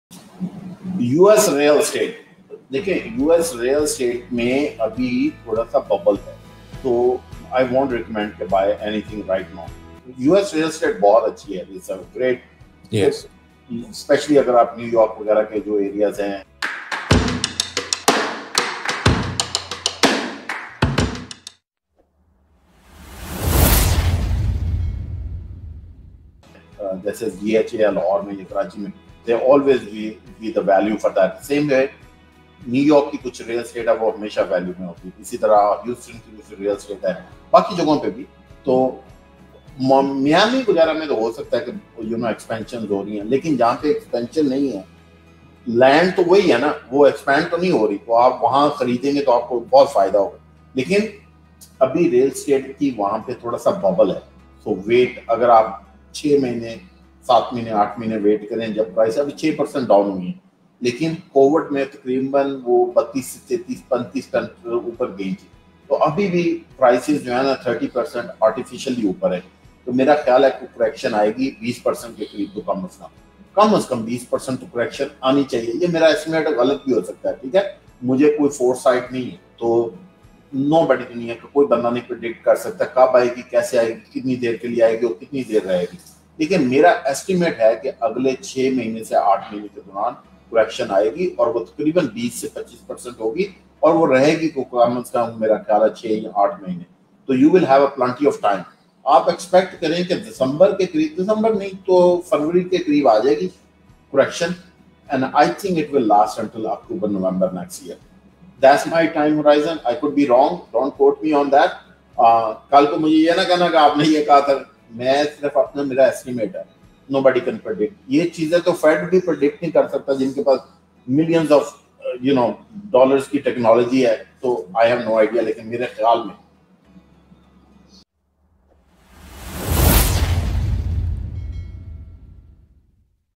Investing In US Real Estate 2022 | You Should Know This | Munawar Abadullah
Before you should invest in US real estate you should watch this video till the end. US real estate latest updates in 2022. You can lose your money if you invest in US real estate in 2022. What you need to know about real estate in America. Which state is best to invest in US real estate.

00:00 US real estate 2022
00:30 New York real estate
00:40 Miami real estate